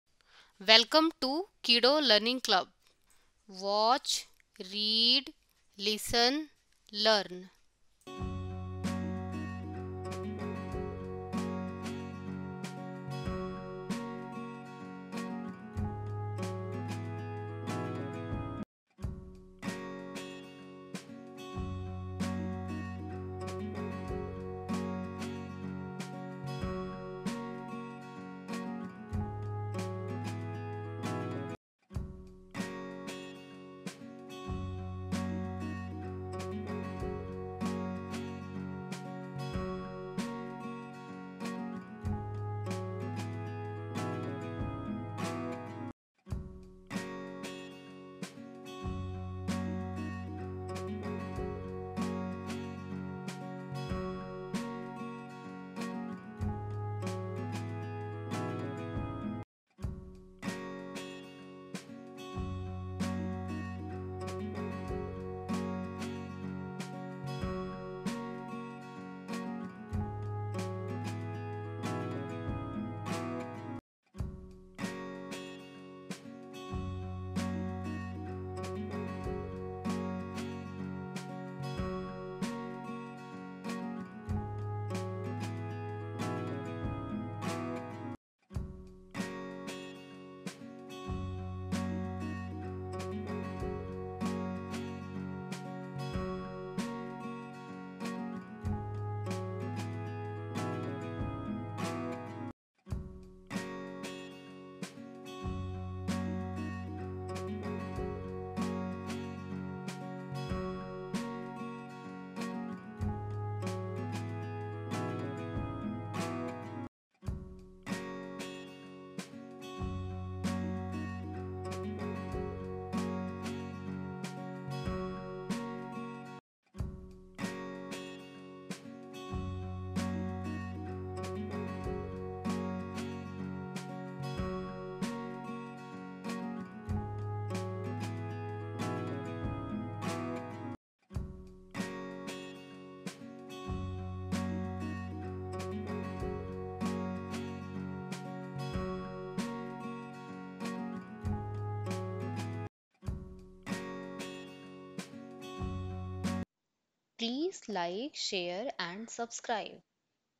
How to make with Building Blocks/Blocks Aeroplane/Blocks Games/Blocks Toys/Blocks/
Lets make an Aeroplane with the help of building blocks. In this video, we will share step by step guide to make the shape as described in the image. This will also help to make some other similar shape